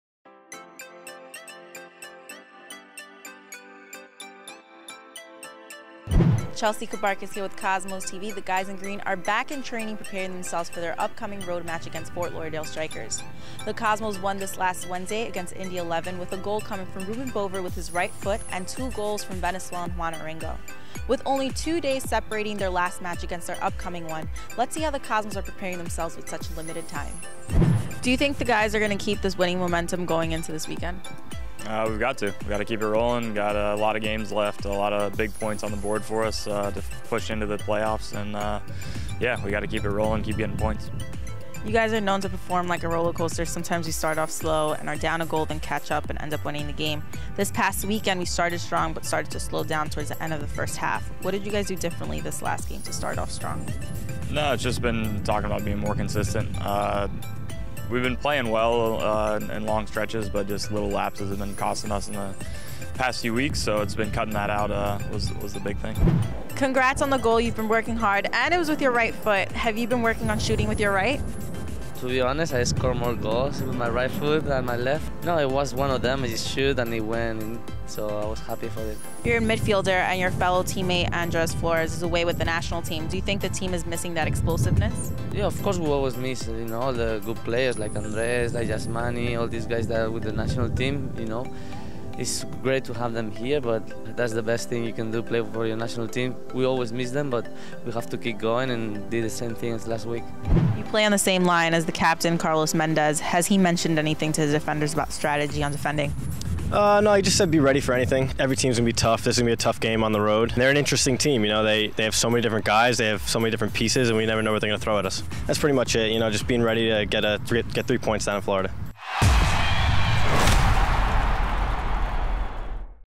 Match Preview: Cosmos Shoot for Fifth Straight Win
The squad looks ahead to a September 3 matchup with the Strikers in Fort Lauderdale.